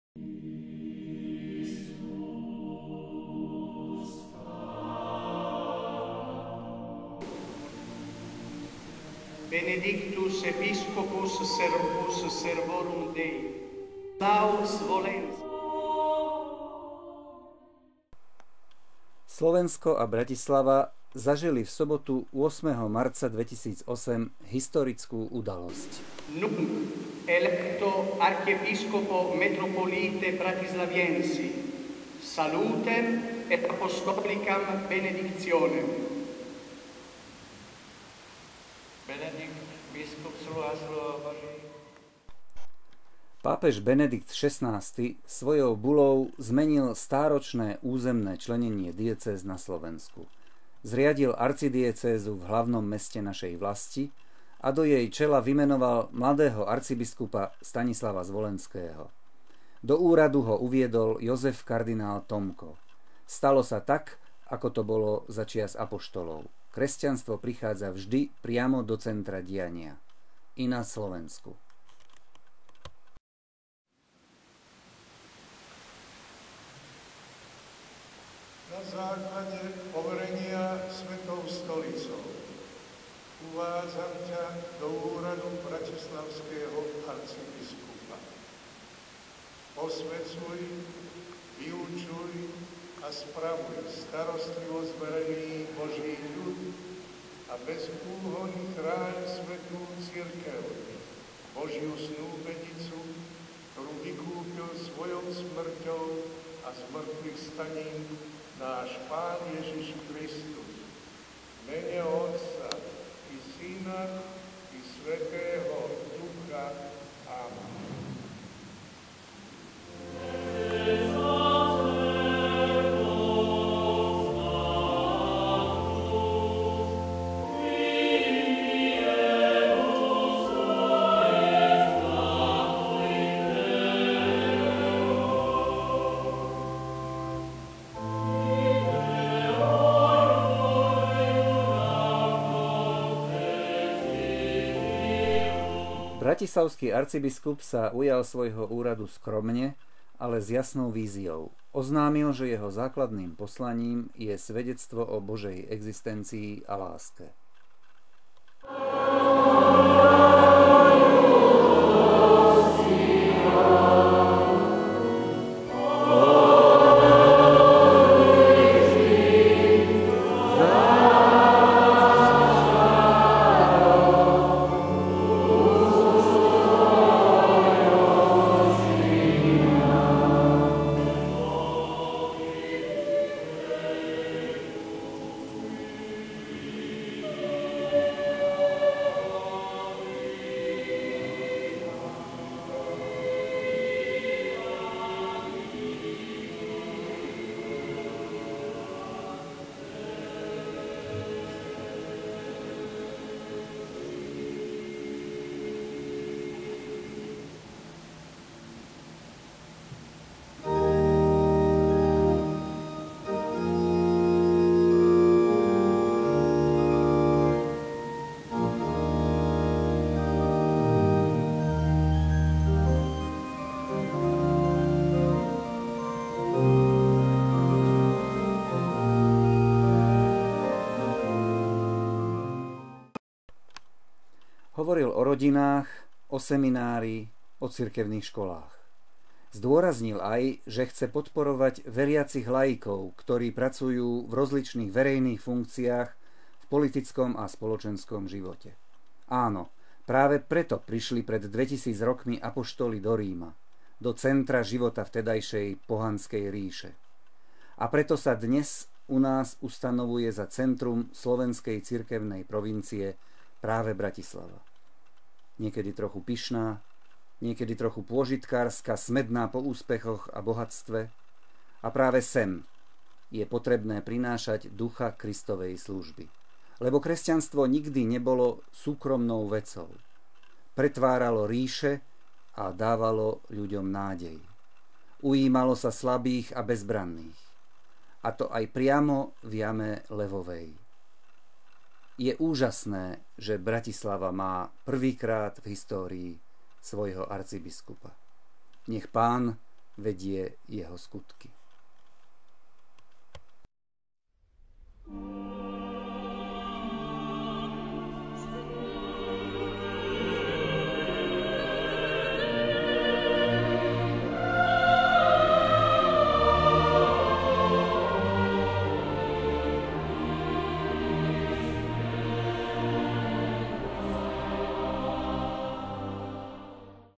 Arcibiskup Zvolenský:Mojim základným poslaním je svedectvo
Slovensko a Bratislava zažili 8.marca 2008 historickú udalosť.